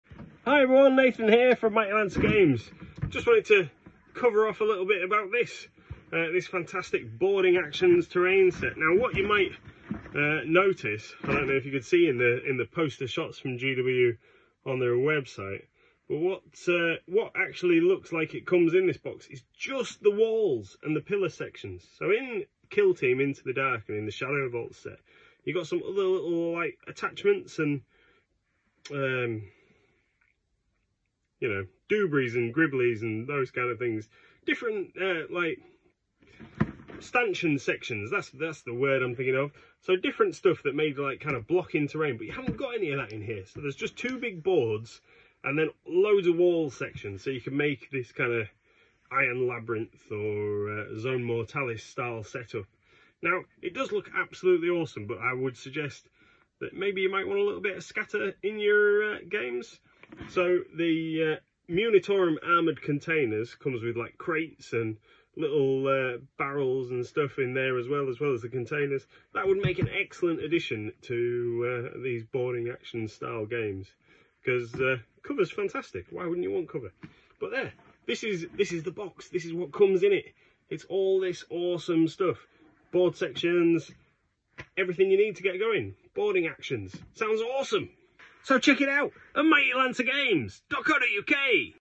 Warhammer 40k Boarding Actions Terrain Set | Inside A Gaming Shop In Bridlington | FLGS
Let’s take a closer look Warhammer 40k Boarding Actions Terrain Set with Mr MLG in the stock room.